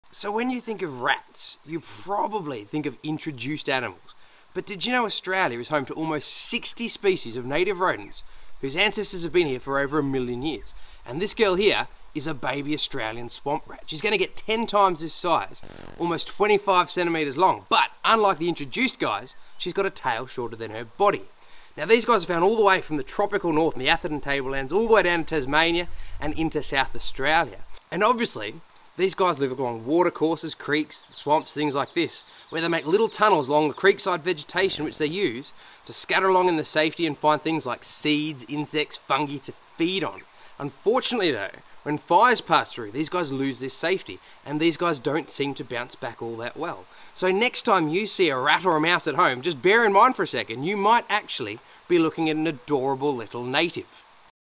Australian Swamp Rat
When most people think of rats they imagine the introduced pests like the black and brown rats, but here in Australia we are home to almost 60 species of native rodents who’s ancestors have been here for at least 1 million years
The animal we are featuring in this is the Australian Swamp rat, a native rodent who lives up the east coast of australia inhabiting the dense foliage alongside creeks, swamps and water ways
They rely on this foliage for cover which they create a complex network of tunnels through to forage and avoid predation
Swamp rats feed mostly on seeds, insects and vegetation and unlike the introduced rats are not considered a pest however they are subject to the detrimental impacts of bushfire
When bushfires destroy their safely tunnels swamp rats are often recorded as absent from the ecosystem for many years after